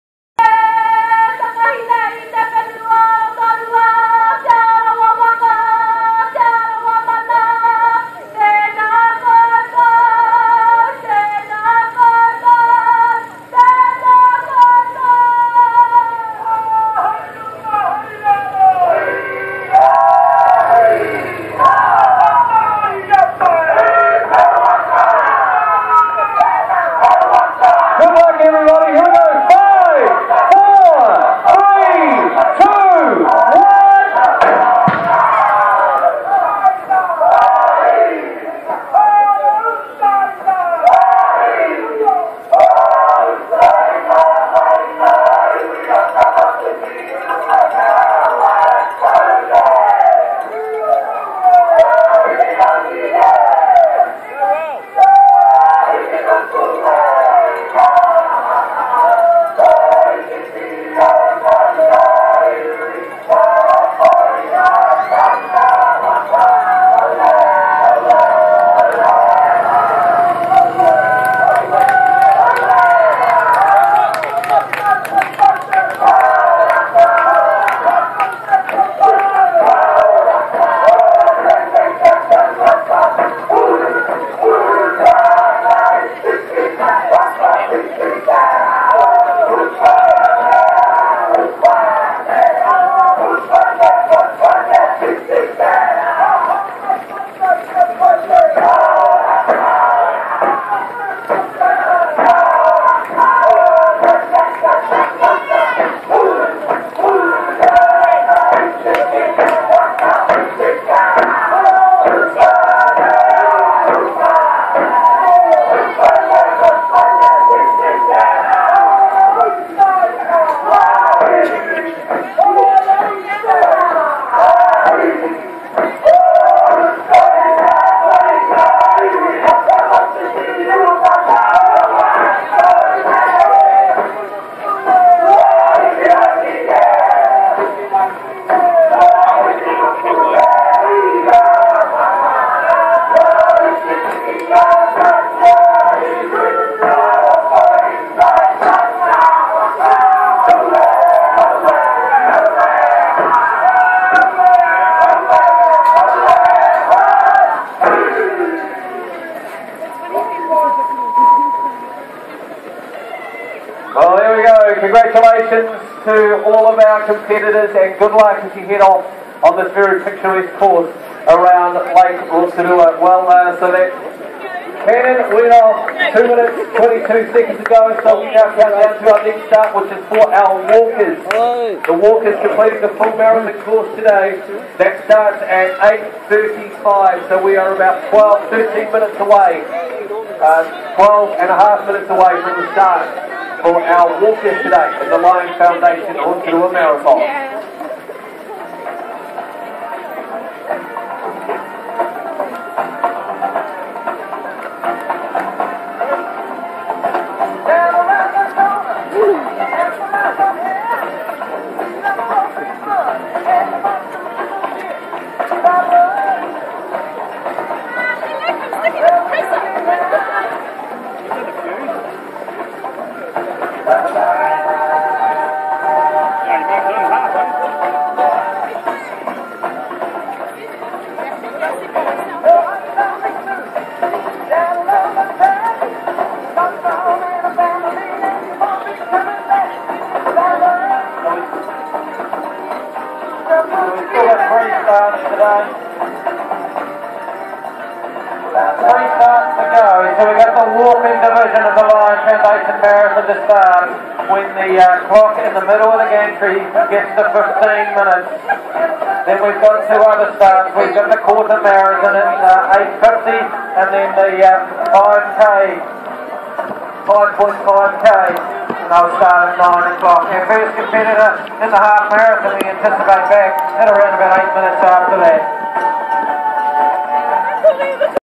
51 st marathon rotorua 2015 runners
3 51 rotorua marathon runners 2015 I took over 110 videos of marathon today and will put them on you tube